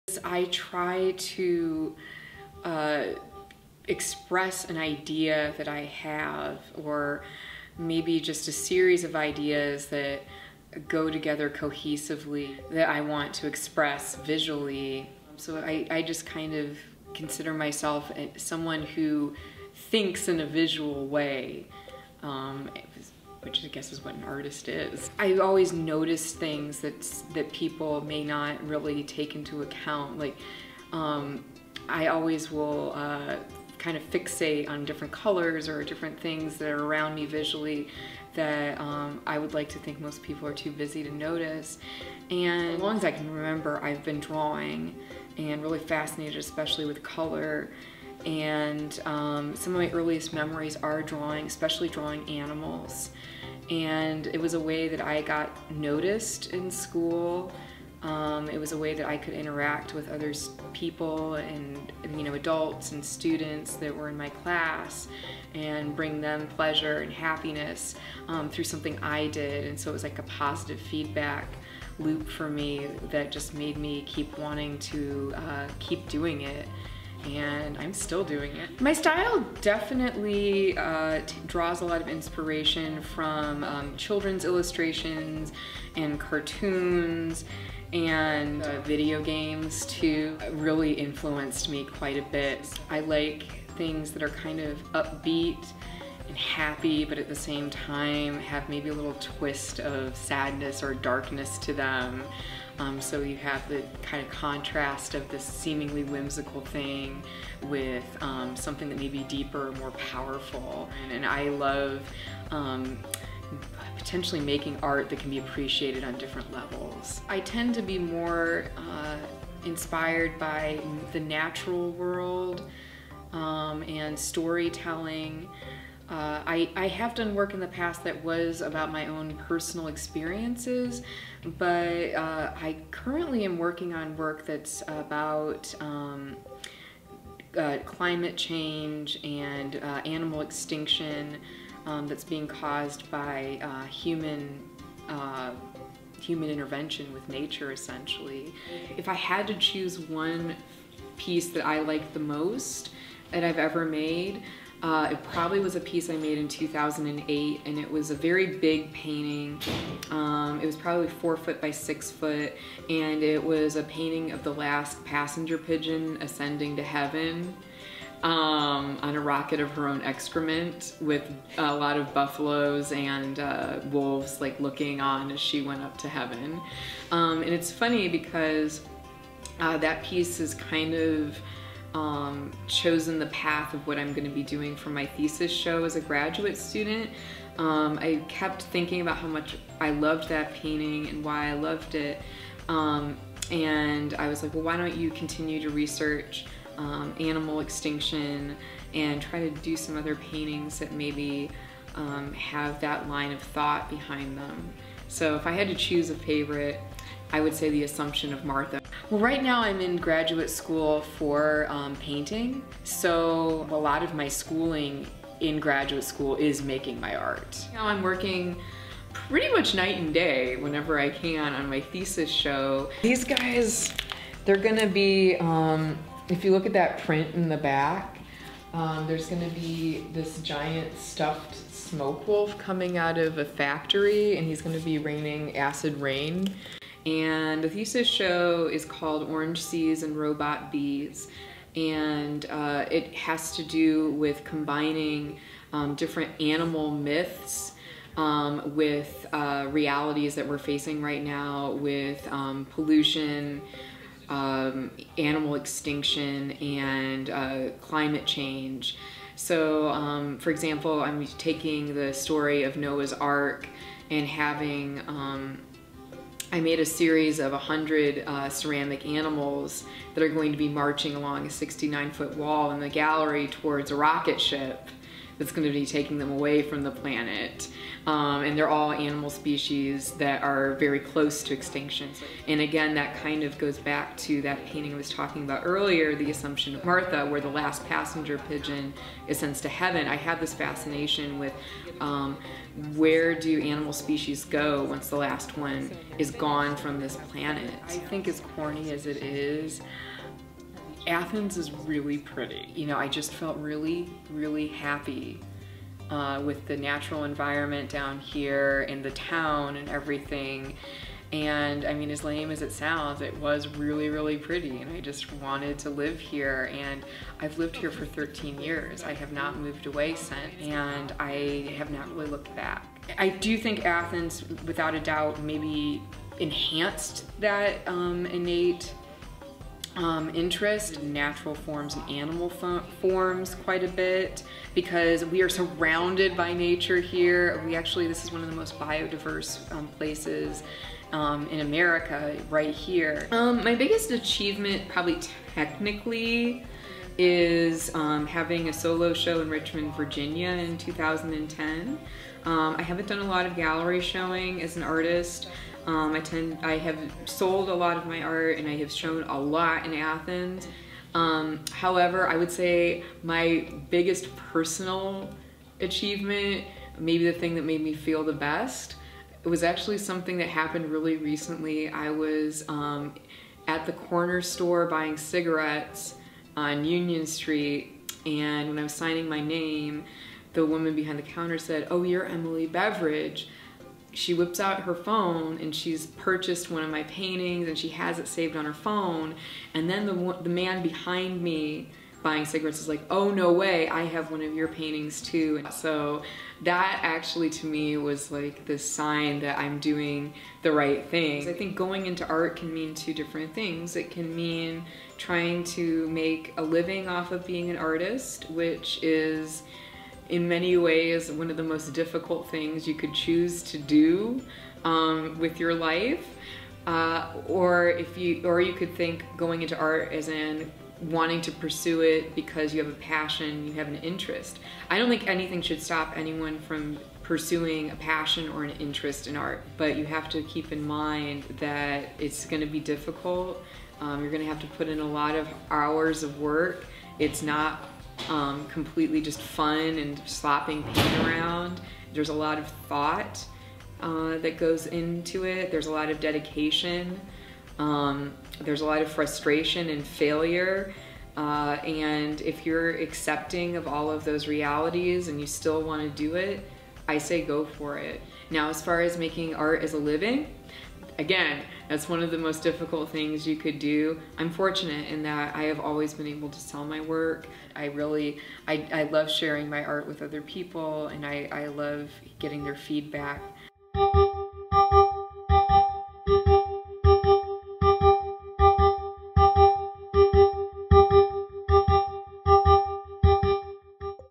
Meet the Artist: Emily Beveridge
On this second installment of WOUB's "Meet the Artist" series, Nicole Schneider talks with Athens, Ohio resident and Ohio University graduate student Emily Beveridge. Beveridge's paintings tend to focus on animals, the environment, and human interaction in an upbeat colorful way with dark symbolic undertones.